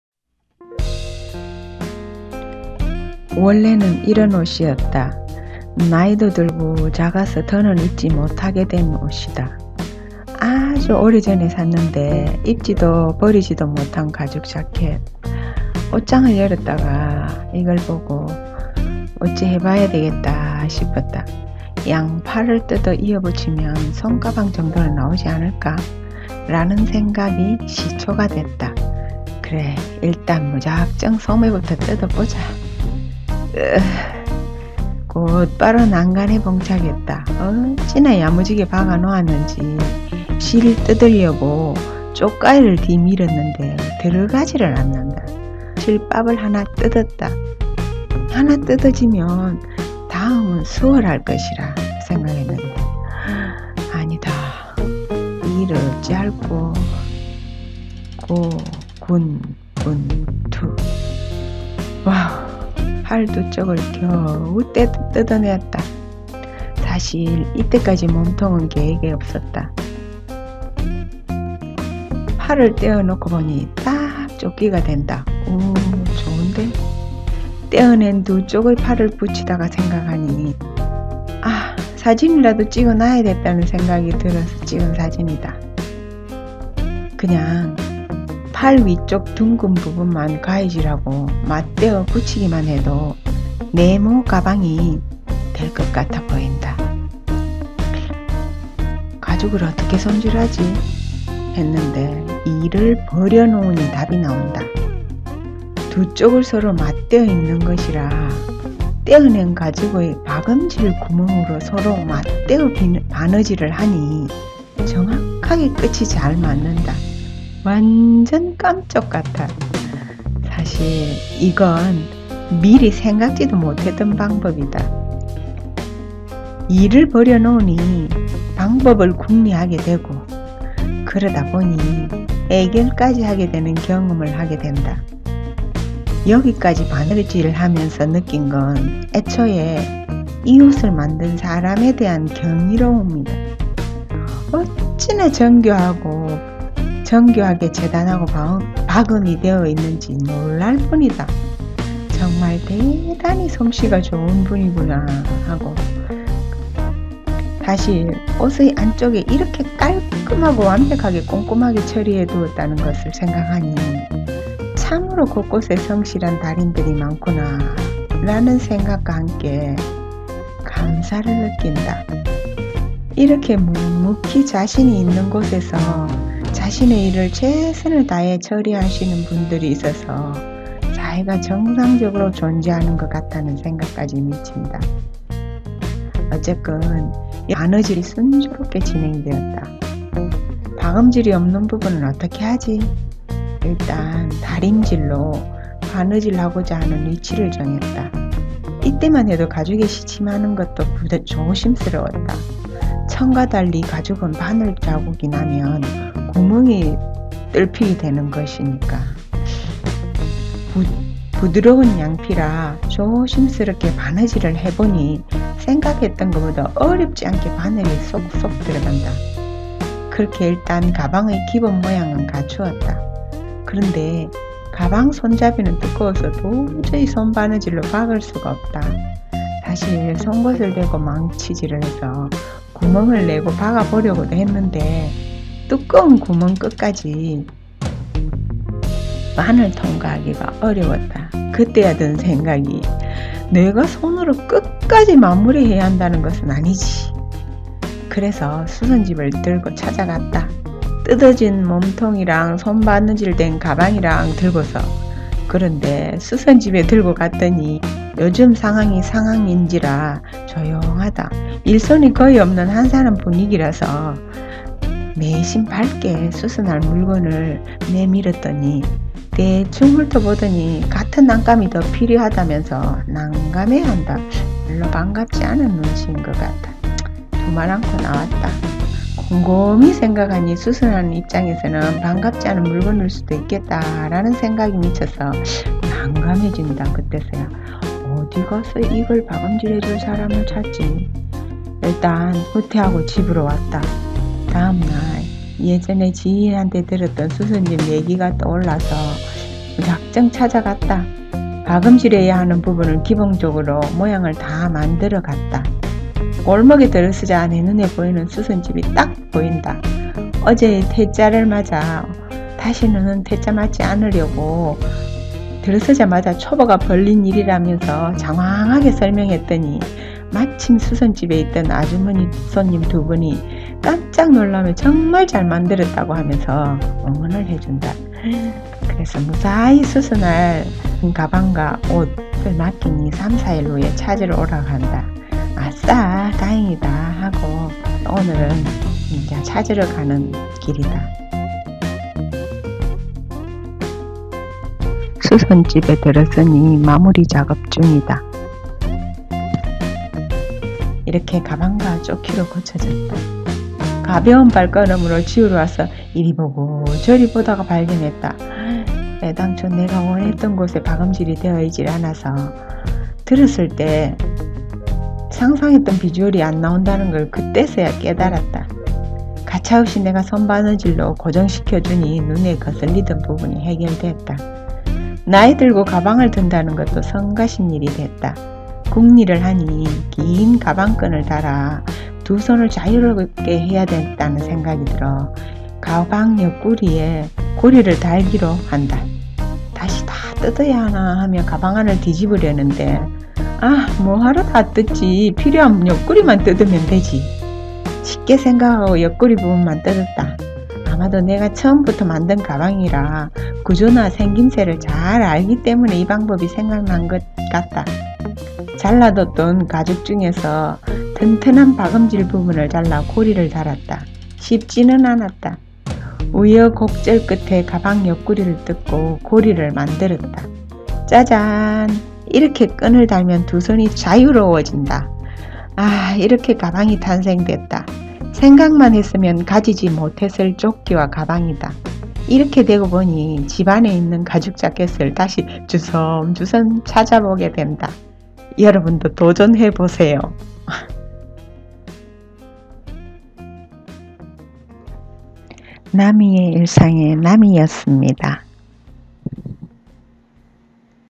가죽재킷을 가죽조끼와 가죽가방으로 셀프수선 #9 Self-repair leather jacket with leather vest and leather bag
불편해서 입지 않는 가죽재킷을 가죽조끼와 가죽가방으로 셀프수선했어요.
이렇게 성공하게 될 줄 몰라서 영상을 처음부터 못 찍었네요.
01:25 가방 만들어진 시초
06:29 긴 가방끈고리달기
Self repairing unused leather jacket with leather vest and leather bag.